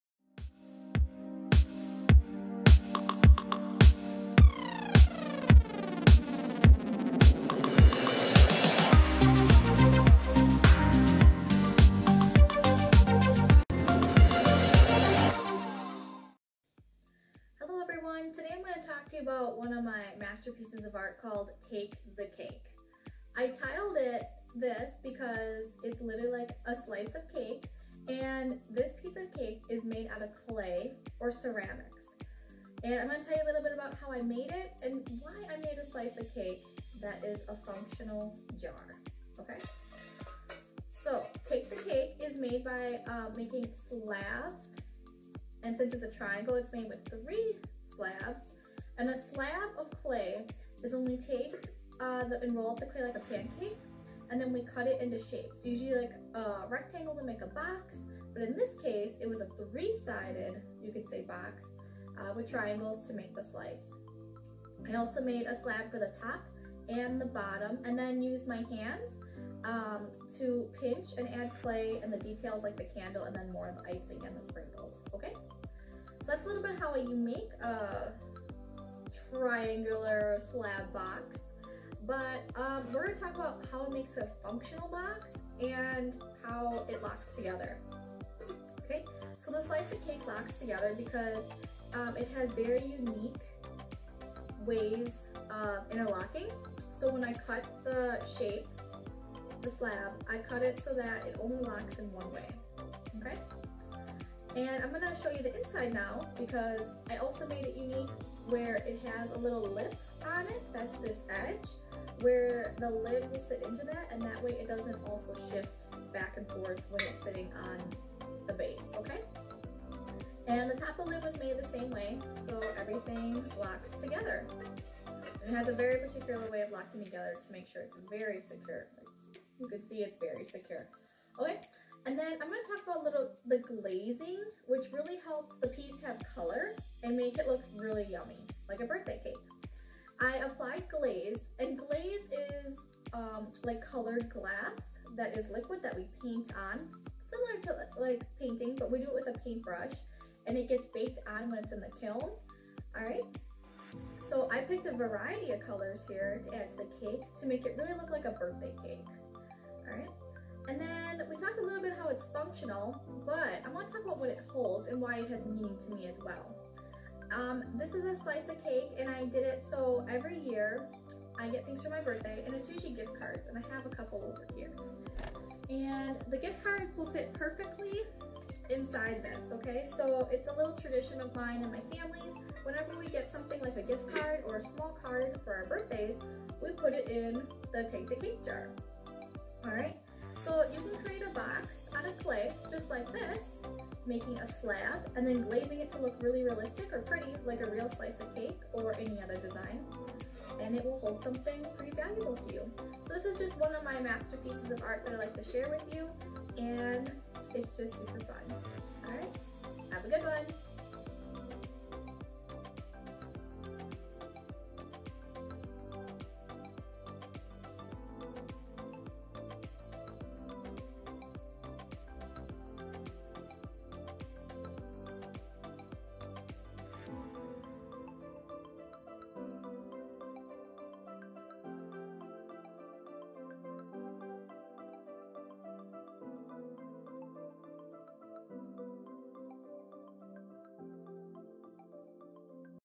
Featured Masterpiece: Take the Cake
Hello Artists!

INSPIRATION FOR THIS ART:
A slice of birthday cake to store some birthday cards or gift cards each year.

YOU WILL LEARN:
 Clay Slab Possibilities
 Locking or securing a lid on a clay box.
 Creativity with materials.